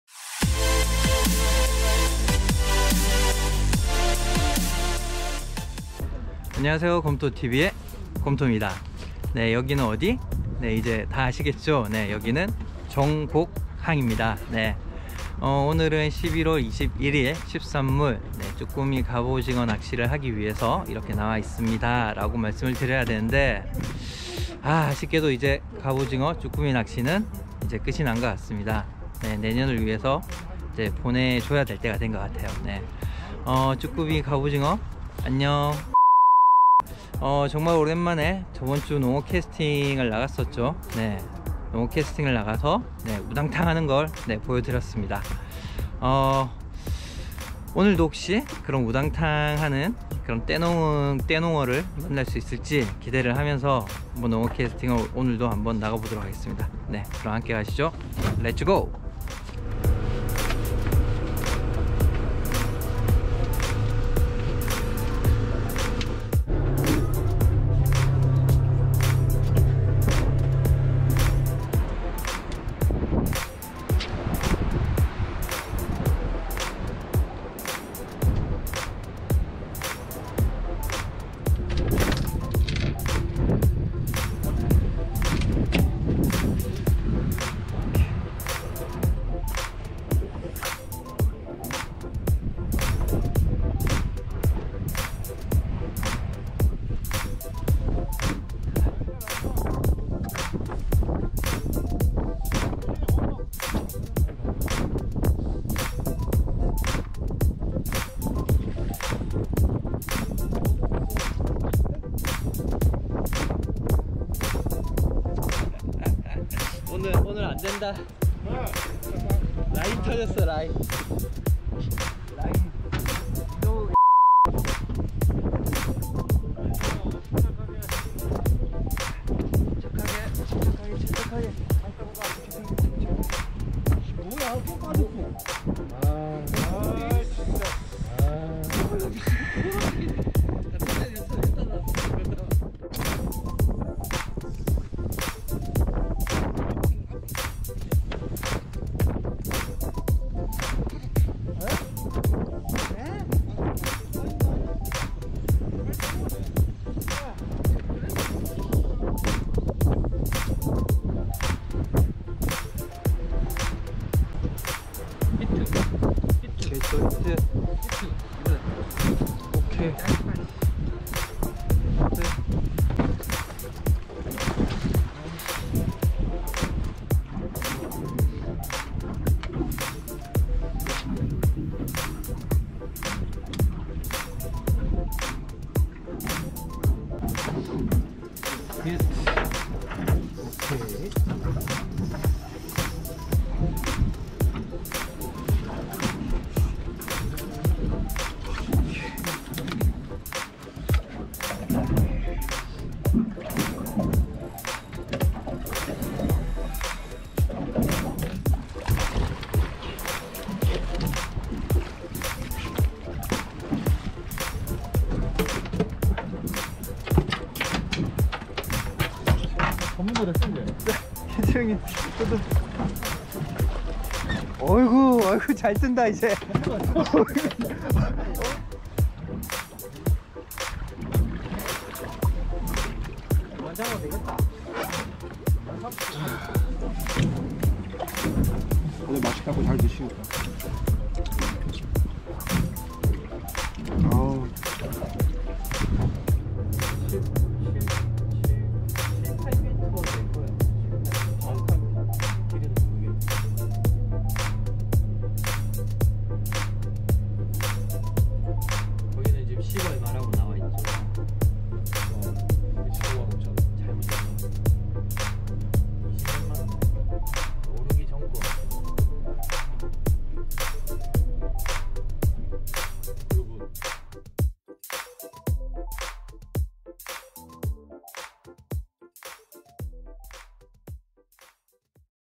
[농어캐스팅/보팅]2주 연속 우당탕 농어캐스팅? 연속 털림으로 따가운 비난과 눈총을 받는 구짜형님... 과연 이 상황을 극복하고 농어를 올릴수있을까?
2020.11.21

저번주에 이어서 다시 농어캐스팅을 나갔다 왔습니다. 저번주와는 전혀 다른 날씨와 농어들의 반응. 한주 사이에 확연히 달라졌습니다.

과연 저번주에 이어서 우당탕 농어 캐스팅이 될지... 아니면 농어가 없는 농어 캐스팅이 될지... 영상으로 확인해보세요.

그럼 영상 재미있게 봐주세요.

촬영장비
카메라 : 고프로8

보트
윙피셔23